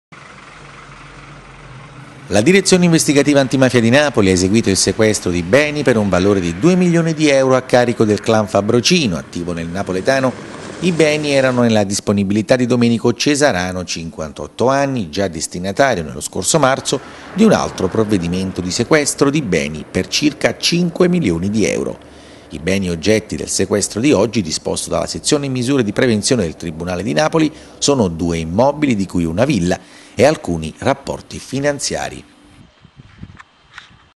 Palma Campania (NA) - Camorra, sequestrati beni per 2 milioni al clan Fabbrocino (09.04.13)
- Palma Campania (NA) - La direzione investigativa antimafia ha eseguito un decreto di sequestro di beni, per un valore di 2 milioni di euro, nei confronti di Domenico Cesarano, 58 anni, di Ottaviano (Napoli), detto "Mimì 'o pezzaro", ritenuto affiliato al clan Fabbrocino. Già lo scorso 14 marzo l'uomo era stato destinatario di un altro provvedimento di sequestro di beni per un valore di 5 milioni di euro. Poi, nel corso dell'indagine, sono emersi ulteriori beni riconducibili a Cesarano: da qui un ulteriore provvedimento emesso dal tribunale di Napoli su richiesta della Direzione distrettuale antimafia partenopea.